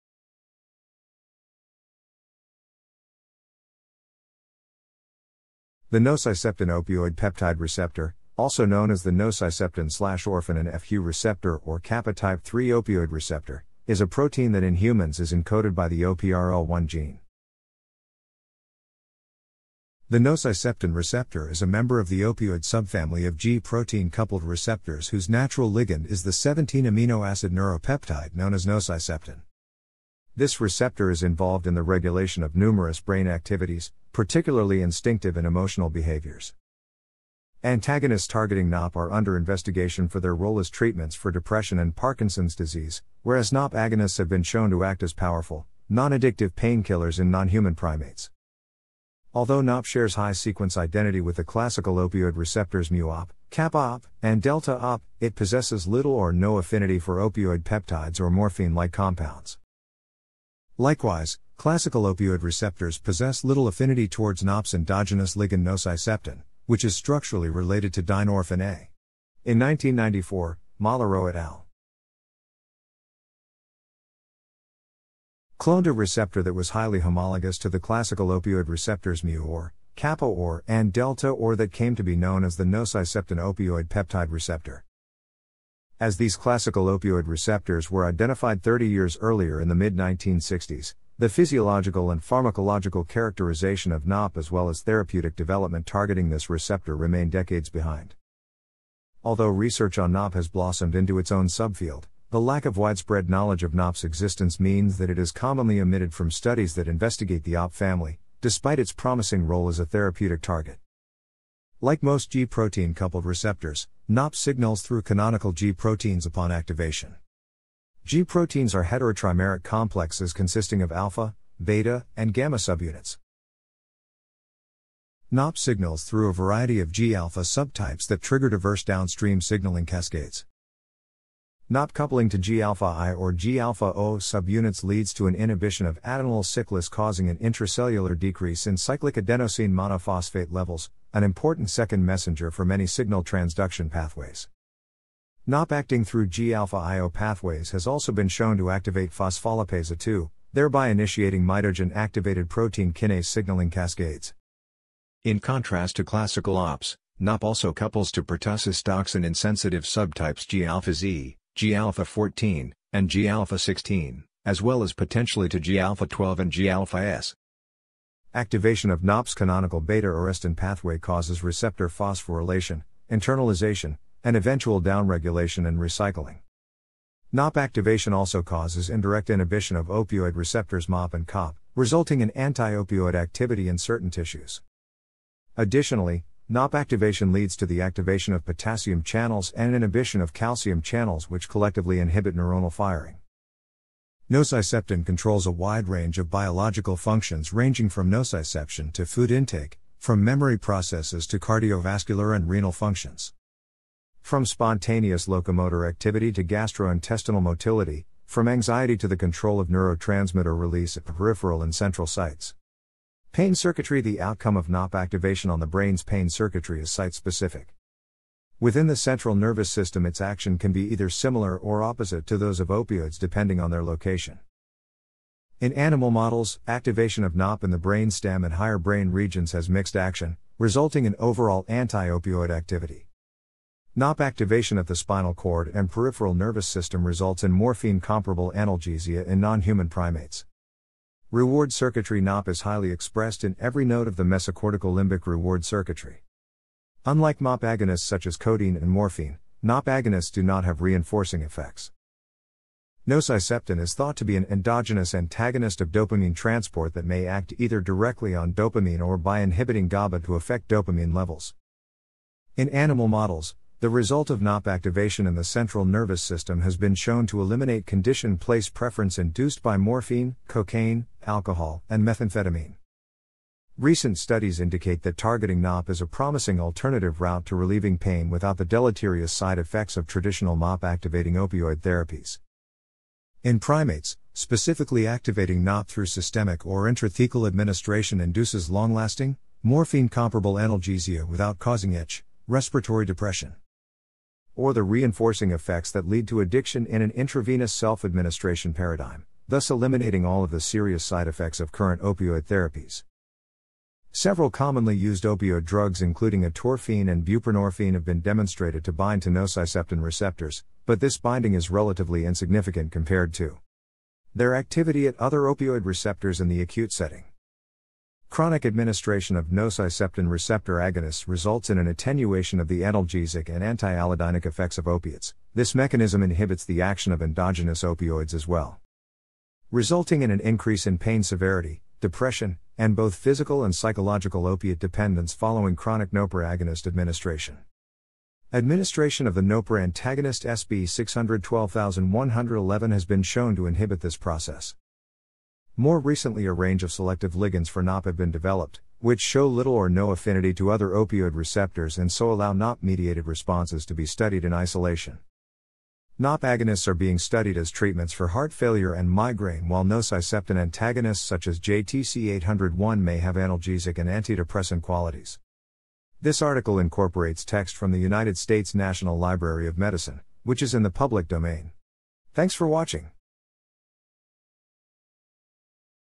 Nociceptin receptor
G_protein-coupled_receptors
The nociceptin opioid peptide receptor （NOP）, also known as the nociceptin/orphanin FQ （N/OFQ） receptor or kappa-type 3 opioid receptor, is a protein that in humans is encoded by the OPRL1 （opioid receptor-like 1） gene.
The nociceptin receptor is a member of the opioid subfamily of G protein-coupled receptors whose natural ligand is the 17 amino acid neuropeptide known as nociceptin （N/OFQ）.
This receptor is involved in the regulation of numerous brain activities, particularly instinctive and emotional behaviors.
Antagonists targeting NOP are under investigation for their role as treatments for depression and Parkinson's disease, whereas NOP agonists have been shown to act as powerful, non-addictive painkillers in non-human primates.
Although NOP shares high sequence identity （~60%） with the ‘classical’ opioid receptors μ-OP （MOP）, κ-OP （KOP）, and δ-OP （DOP）, it possesses little or no affinity for opioid peptides or morphine-like compounds.
Likewise, classical opioid receptors possess little affinity towards NOP's endogenous ligand nociceptin, which is structurally related to dynorphin A. In 1994, Mollereau et al.
cloned a receptor that was highly homologous to the classical opioid receptors （OPs） μ-OR （MOP）, κ-OR （KOP）, and δ-OR （DOP） that came to be known as the Nociceptin Opioid Peptide receptor （NOP）.
As these "classical" opioid receptors were identified 30 years earlier in the mid-1960s, the physiological and pharmacological characterization of NOP as well as therapeutic development targeting this receptor remain decades behind.
Although research on NOP has blossomed into its own sub-field, the lack of widespread knowledge of NOP's existence means that it is commonly omitted from studies that investigate the OP family, despite its promising role as a therapeutic target.
Like most G-protein coupled receptors, N...